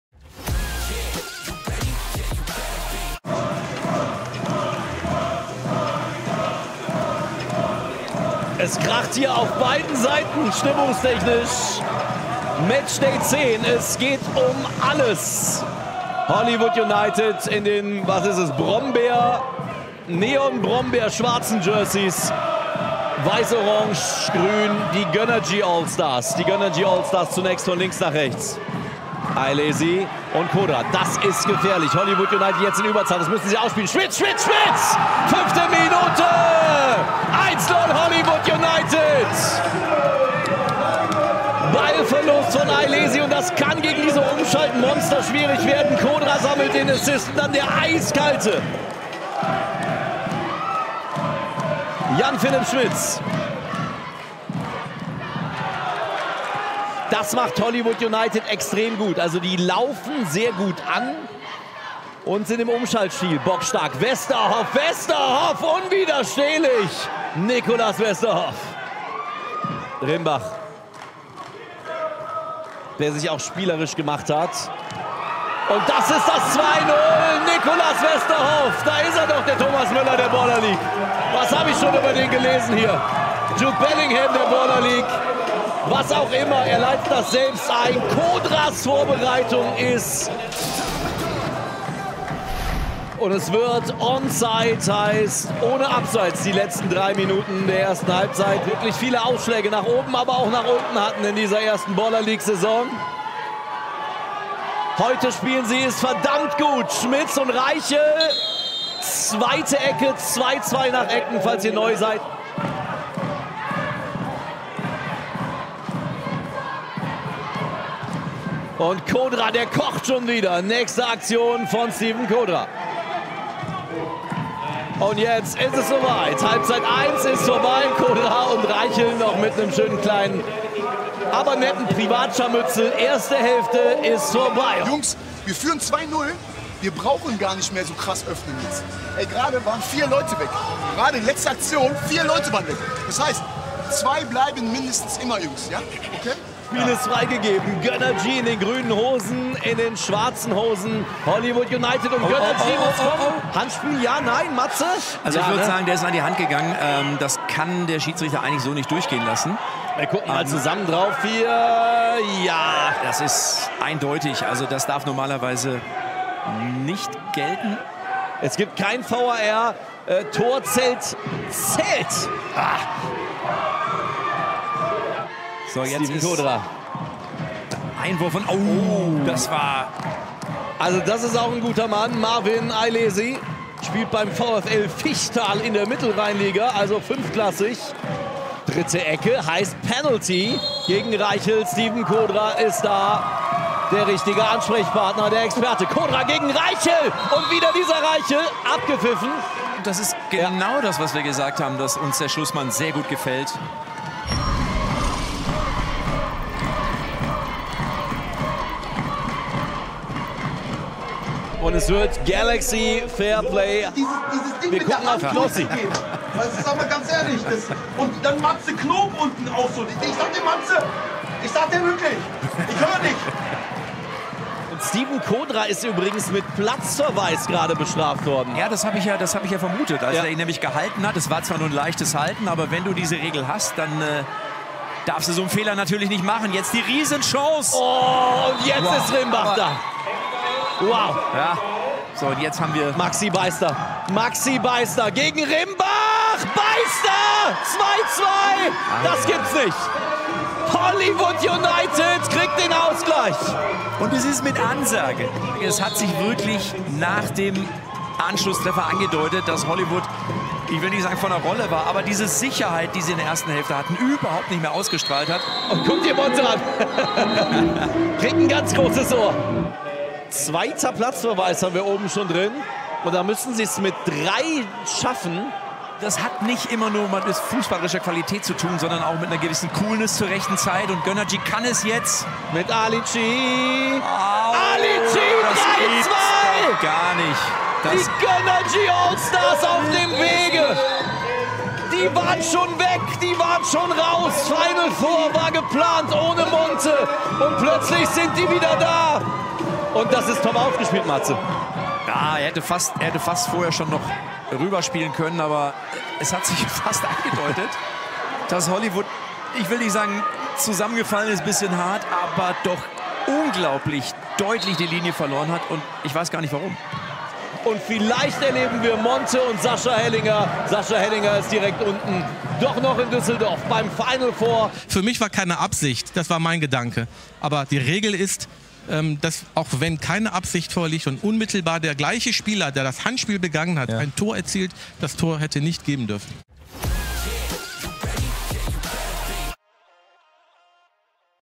Knossi vs. Monte! | Gönrgy Allstars vs. Hollywood United | Highlights Baller League | Gameday 10
Viel Spaß mit den Highlights des Spiels Gönrgy Allstars gegen Hollywood United an Gameday 10 der Baller League!

Welcome to a new era of football! ⚽️
Hier verpasst ihr nichts!
Danke an unsere Partner!